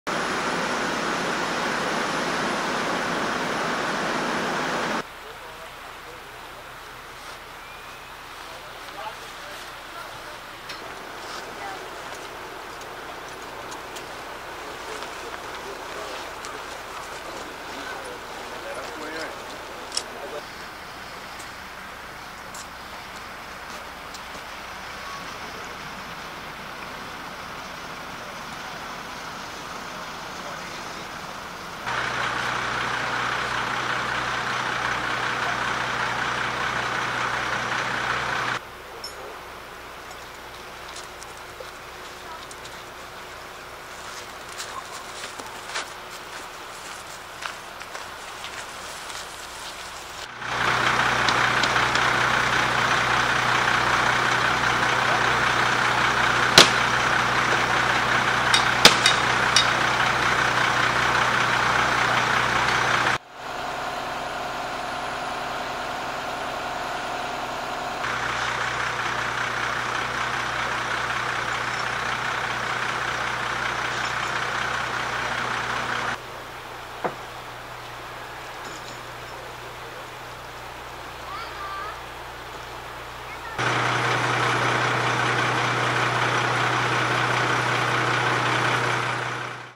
River Search
Kingston fire department requested mutual aid from Annapolis Royal for their water rescue team to search the area after a Mazda pickup truck was spotted in the Fales River in Greenwood, Nova Scotia April 19. RCMP also called in a canine unit. Police located the driver a short time later.